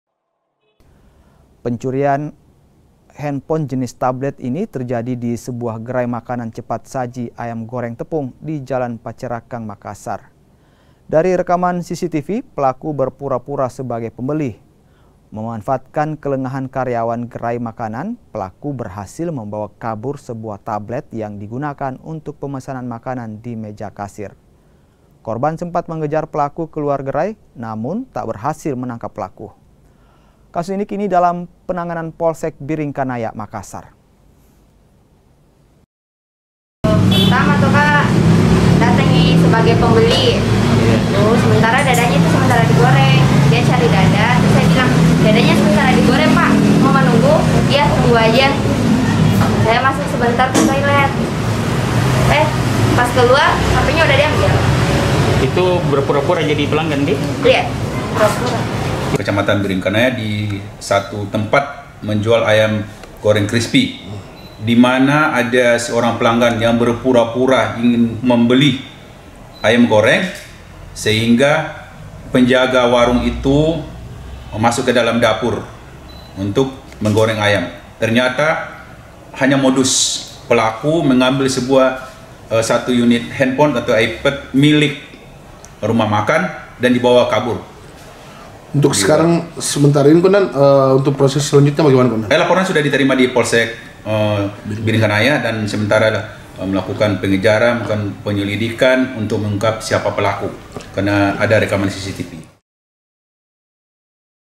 Pencurian Di Gerai Makanan Cepat Saji Terekam CCTV
MAKASSAR, KOMPAS.TV - Aksi pencurian dengan modus sebagai pembeli berhasil membawa kabur handphone korbannya di gerai makanan cepat saji di makassar. Aksi pelaku pun terekam kamera pemantau atau CCTV.

Aksi pencurian handphone jenis tablet ini terjadi di sebuah gerai makanan cepat saji ayam goreng tepung di jalan pacerakkang makassar. Dari rekaman cctv pelaku berpura pura sebagai pembeli.

Memanfaatkan kelengahan karyawan gerai makanan pelaku berhasil membawa kabur

Sebuah tablet yang digunakan untuk pemesanan makanan di meja kasir. Korban sempat mengejar pelaku ke luar gerai namun tak berhasil menangkap pelaku. Kasus ini kini dalam penanganan polsek biringkanaya makassar.